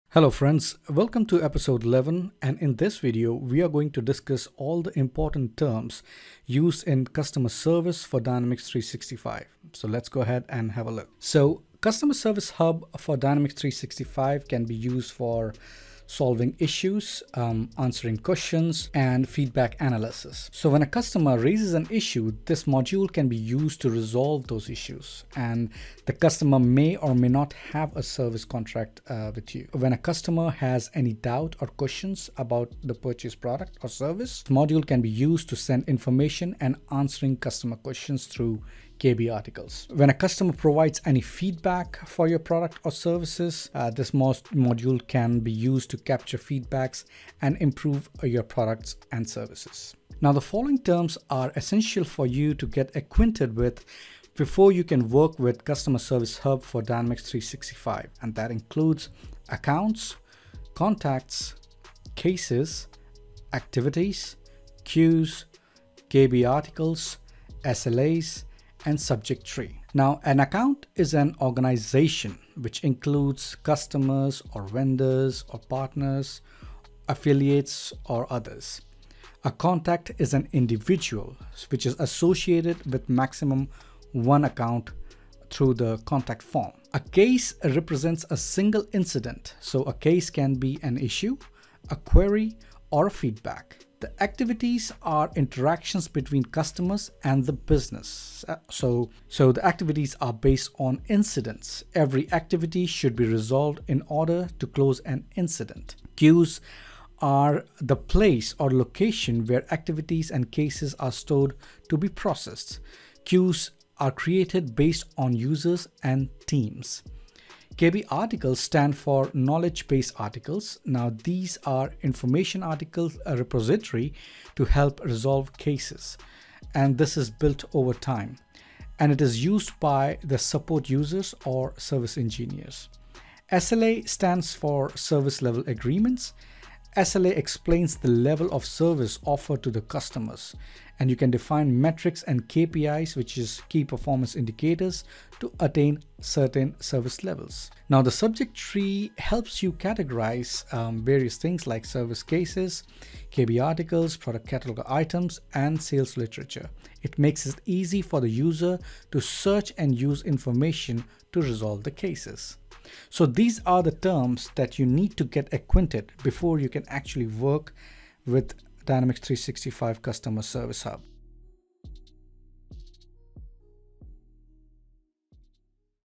EP11 Important Terms In Customer Service for Dynamics 365
Want To Use Neural AI To Naturally Voice Over Your Scripts?
Episode 11
This video explains all the important terms that you need to get acquainted before you start working with Customer Service Hub for Dynamics 365.